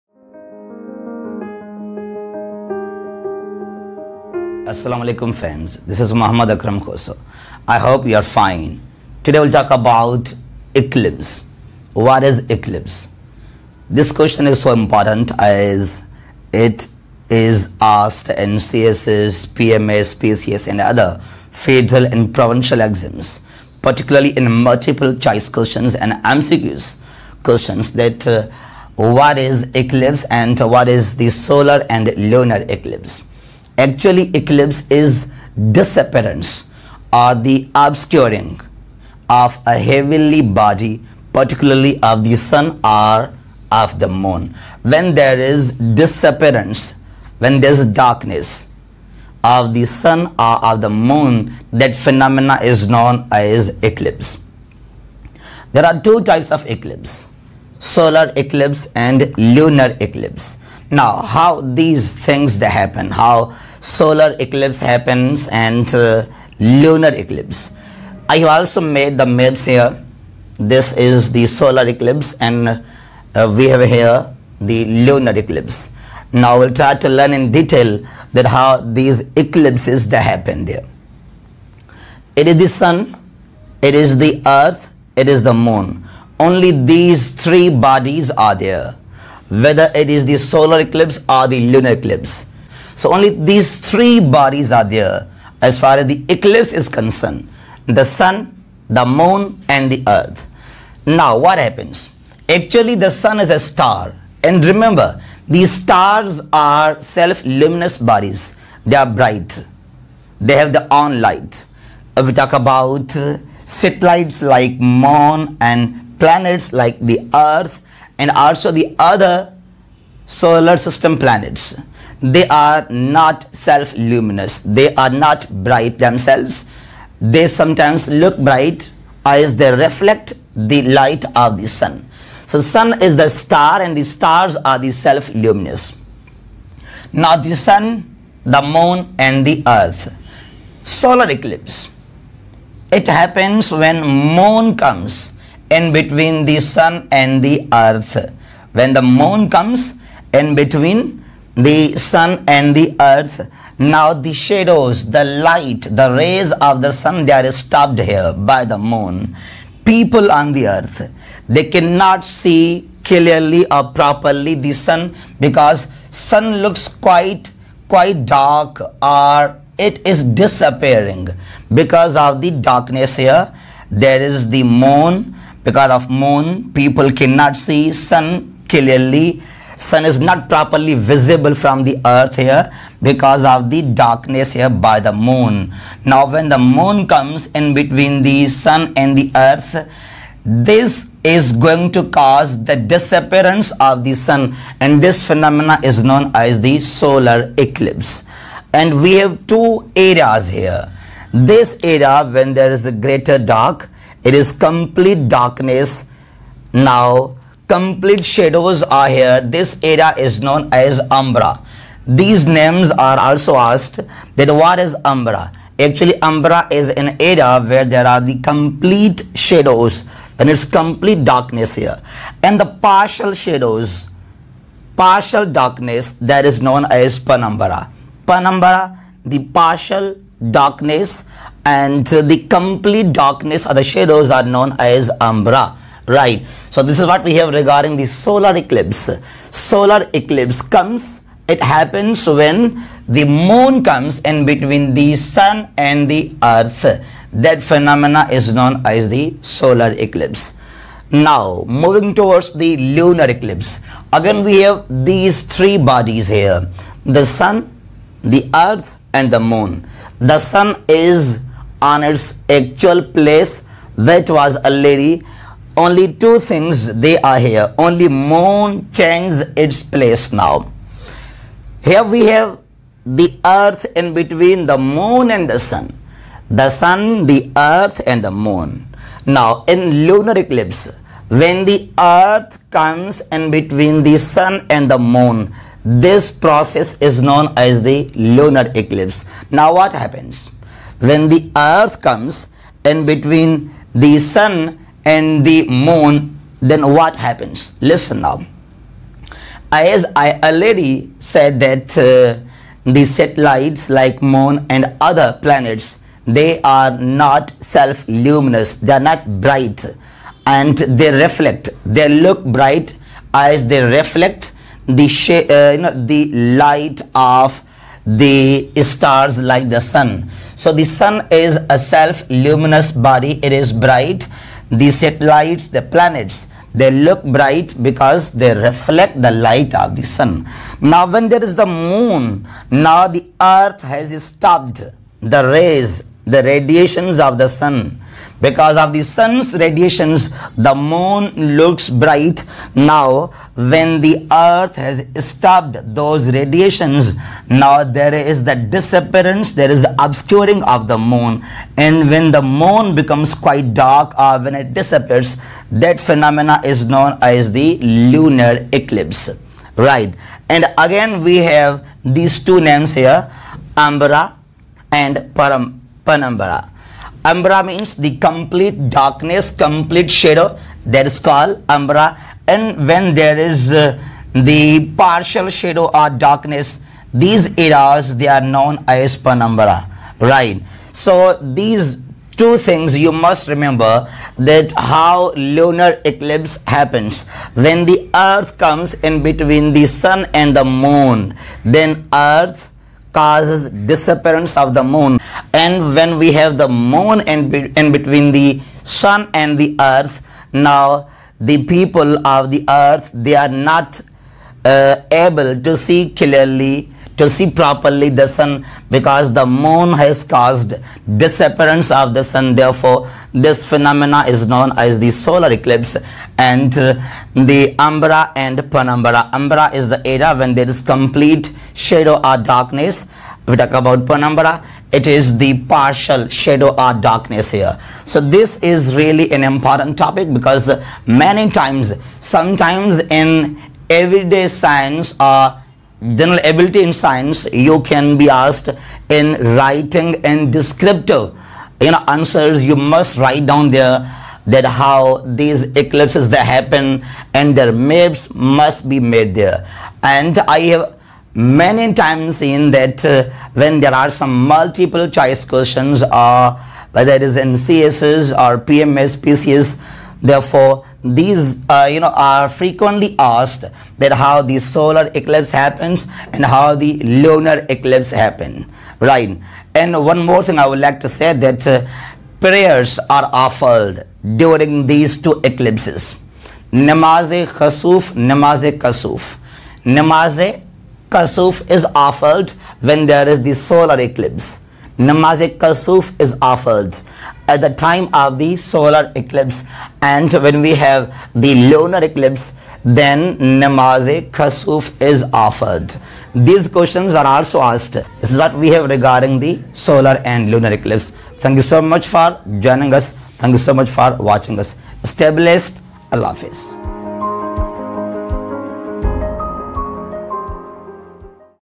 Eclipse | Solar and Lunar Eclipses | by Muhammad Akram
Eclipse is the disappearance of heavenly body, particularly the Sun or the Moon. The solar eclipse occurs when the moon comes in between the earth and the sun. The lunar eclipse happens when the earth comes between the sun and the moon.